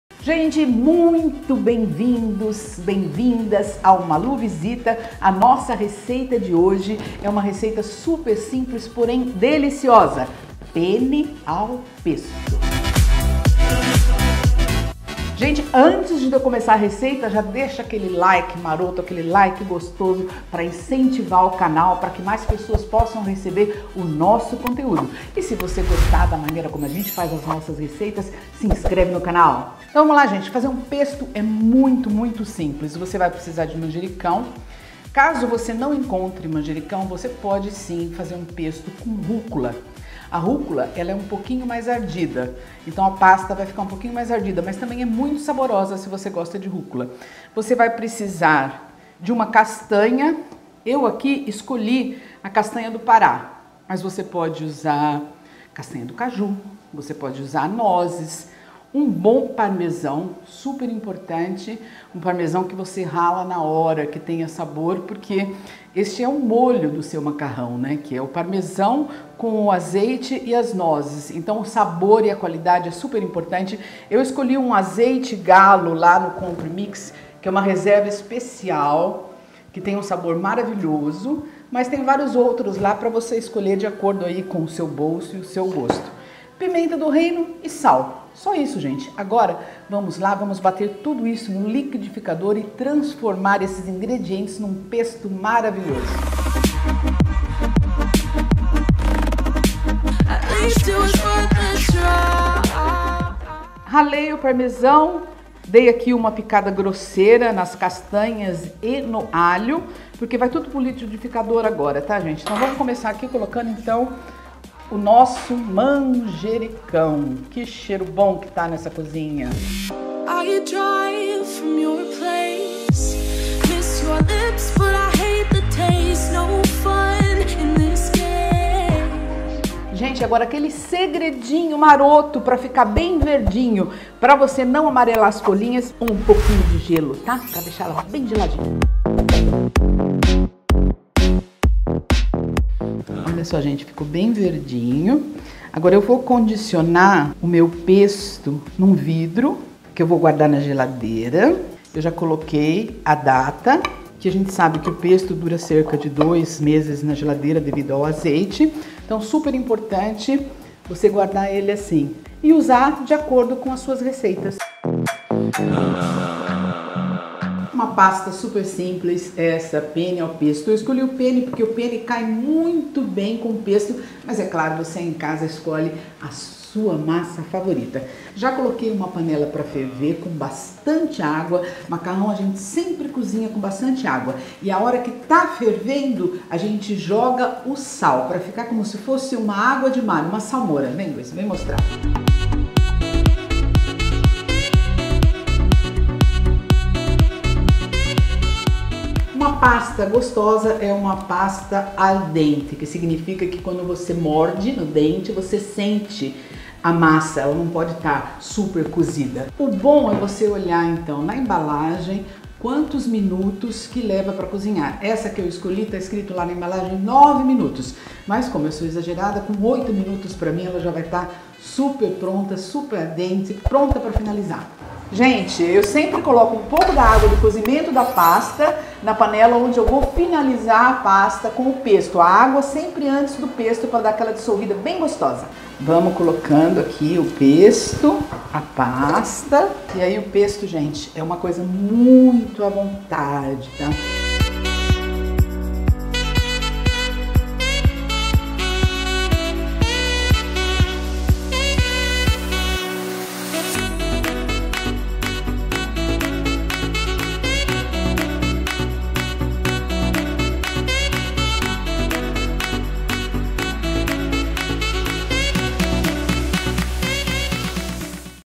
Receita de Penne ao Pesto, macarrão com molho pesto
O molho pesto é um molho italiano de manjericão que combina muito bem com macarrão.
O clássico preparo italiano é grande aliado nos dias de preguiça total. A ideia é sempre ter um pote com pesto na geladeira, aí é só cozinhar o macarrão e o jantar está resolvido em coisa de 20 minutos.

4 dentes de alho
1 colher (chá) de sal
pimenta-do-reino à gosto
1 xícara (chá) de folhas de manjericão, fresco
3 colheres (chá) de castanhas
100 g de queijo parmesão ralado
1 xícara (chá) de azeite

Você pode dobrar a receita e assim ter um pesto para guardar na geladeira. Fica bom com massas, mas também, com peixes e até nas saladas.